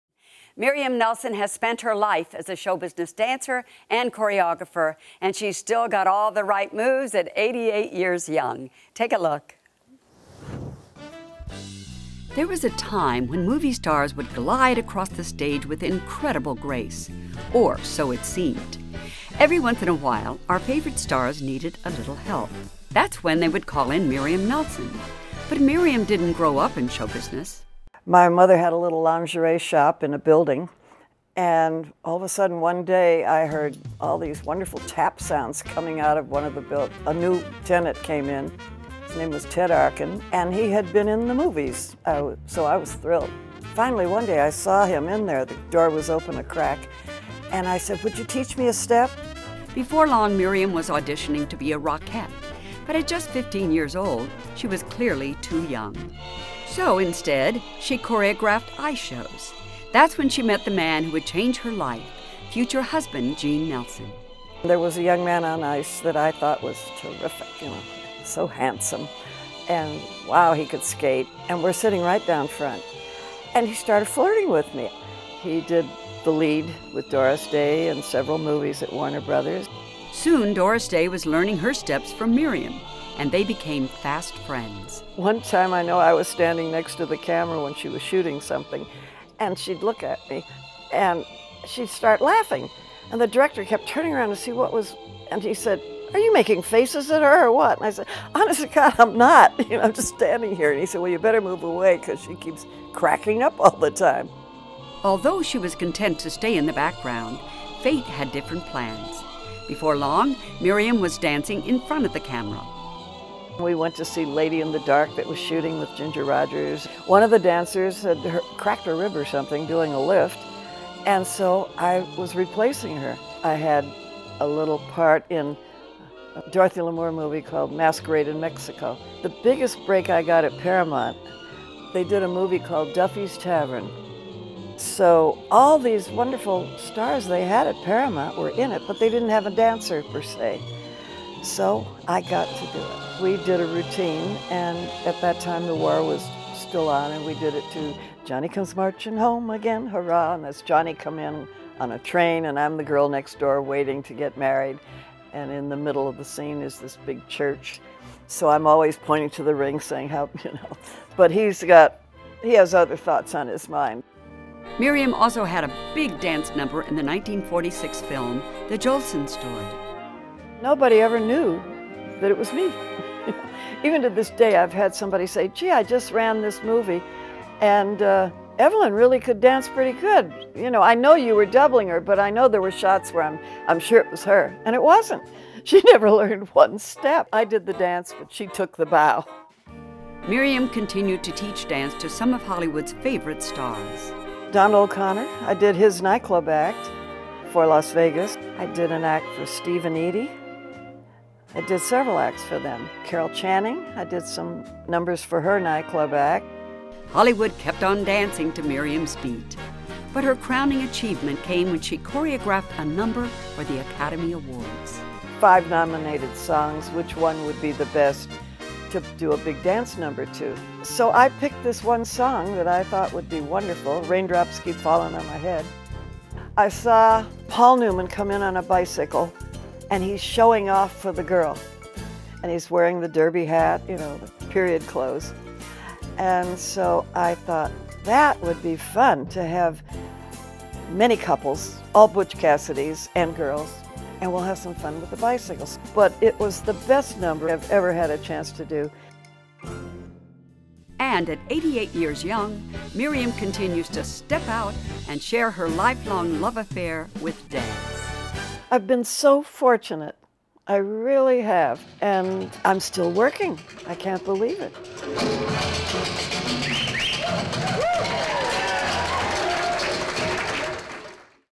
Miriam Nelson
At the Age of 15, choreographer Miriam Nelson found herself telling Golden Age of Hollywood stars what to do... if they wanted to look good dancing on camera. From being Doris Day's best gal pal to understudying her way onto the silver screen, Nelson found herself dancing amongst the stars.